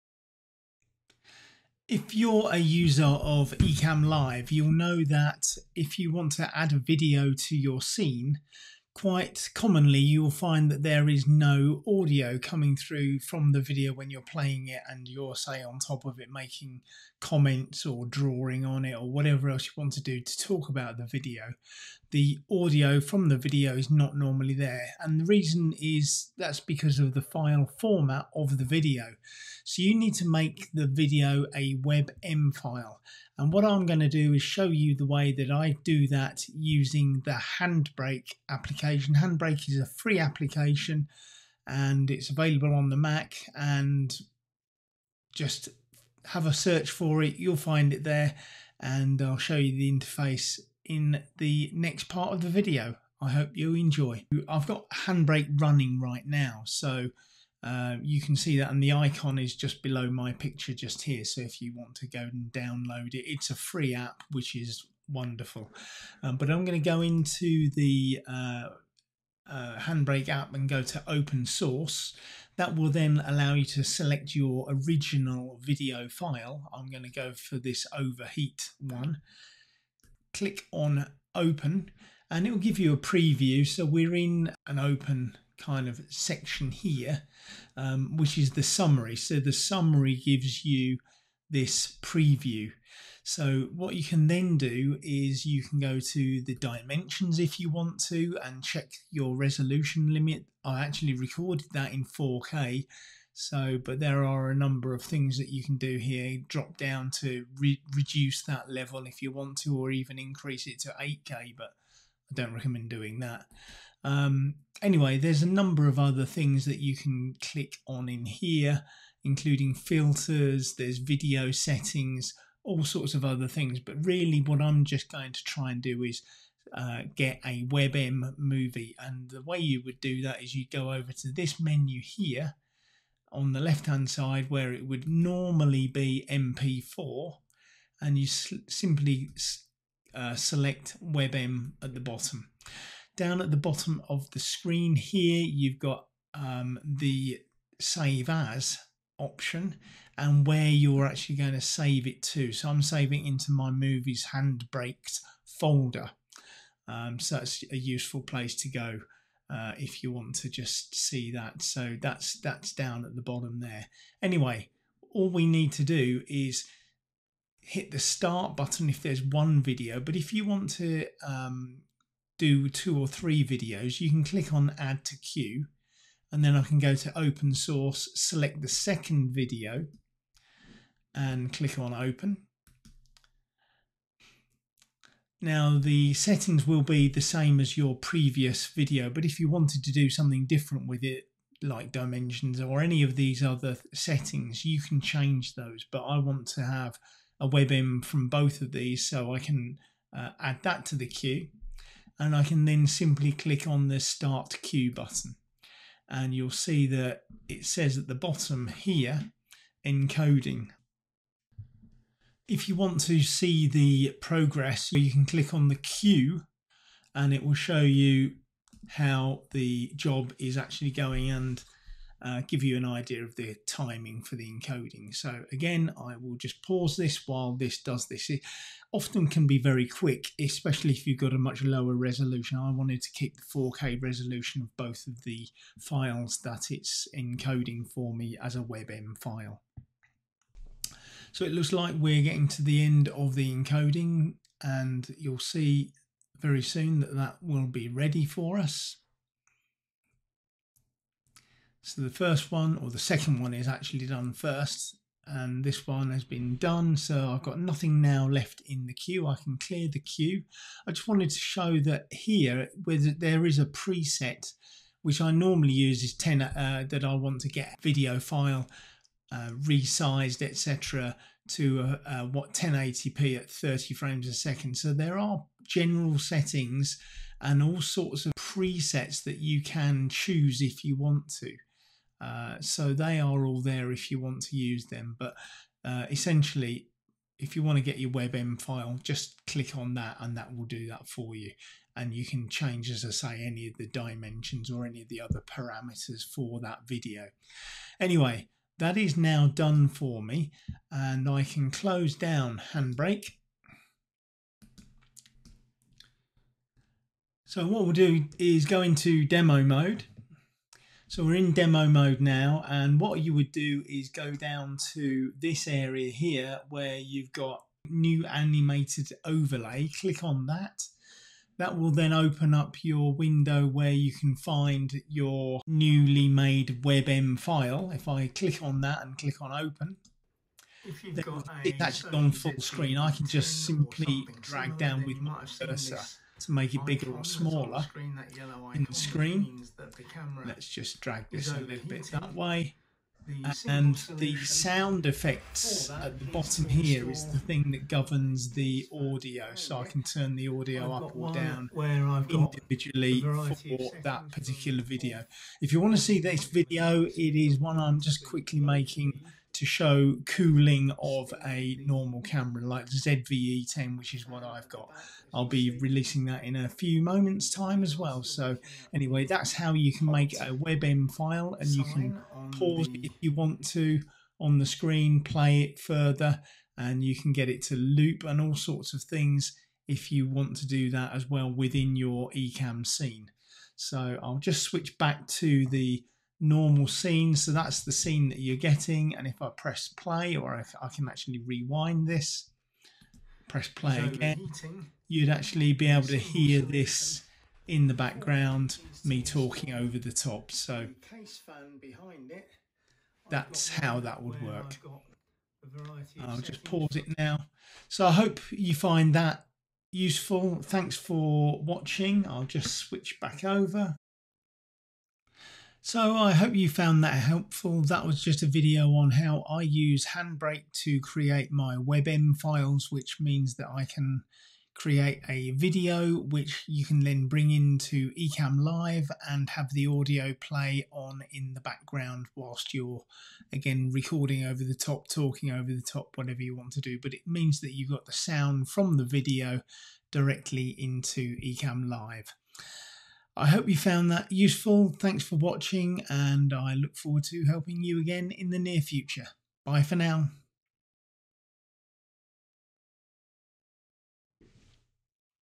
HandBrake for making WebM files
In this video I show you how to make a WebM video file using the FREE HandBrake app on a Mac. This is very useful if you want to show a video in an Ecamm Live "Scene', as a WebM file also includes the ability to play the audio track, something which is not available with most other video file formats.

🎥Ecamm Live - Used to Record or Live Stream all of my videos - follow this link for a free 14-day trial